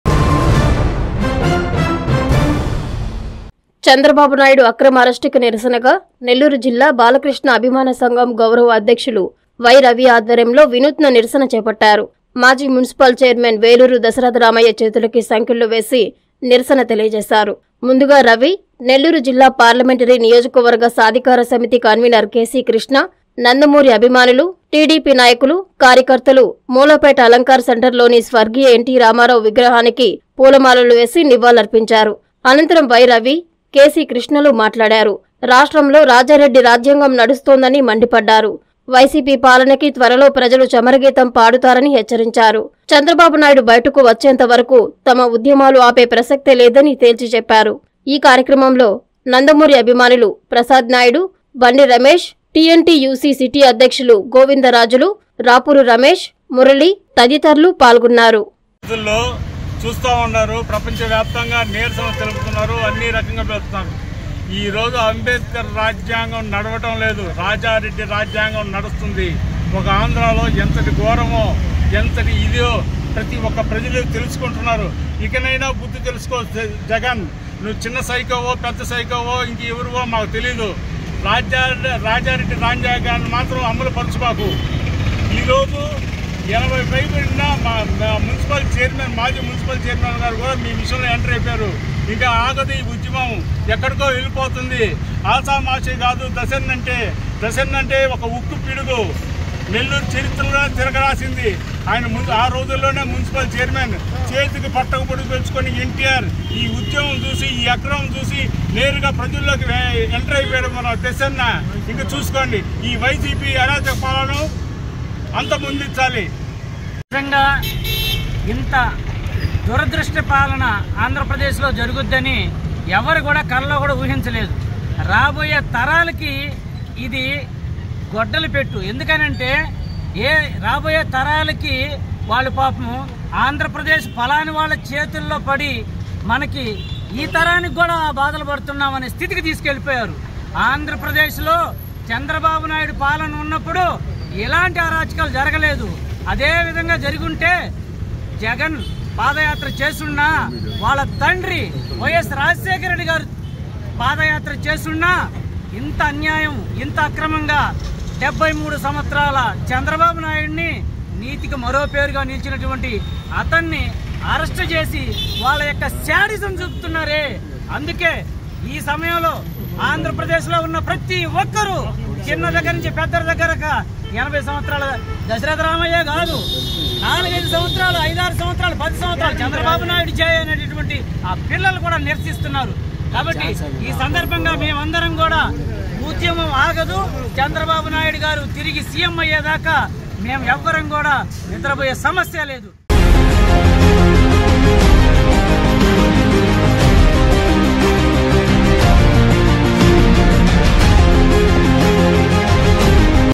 సంకెళ్ల‌తో వినూత్న నిర‌స‌న‌ | Protests on Chandrababu Naidu Arrest | Balakrishna Fans
చంద్ర‌బాబు బ‌య‌ట‌కు వ‌చ్చే వ‌ర‌కు ఉద్య‌మం ఆగ‌దు